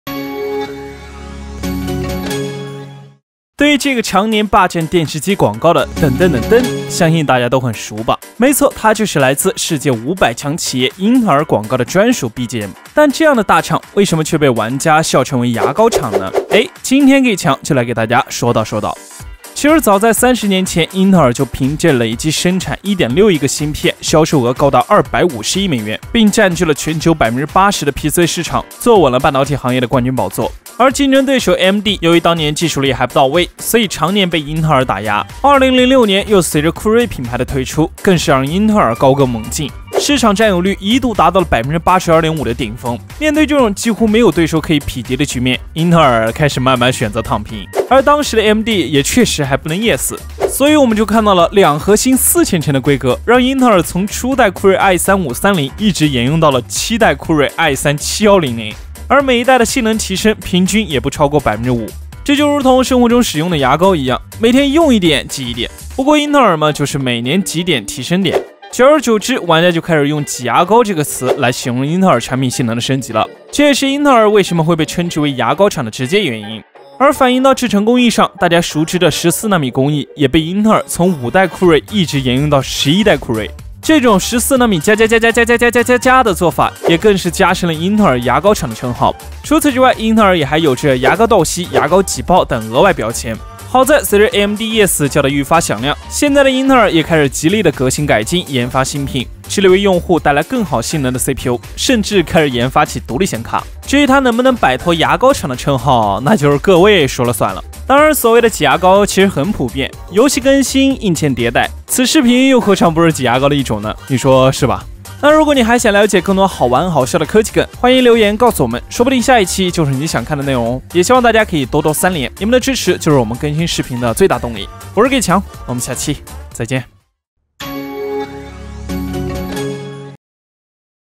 Intel爲什麽被稱爲“牙膏廠”？「超極氪」
“當牙膏擠習慣的時候，14nm就壹直 ”
“但當牙膏擠爆的時候，14nm就變成10nm”
如果妳還想了解更多好玩、好笑的科技梗，歡迎留言告訴我們！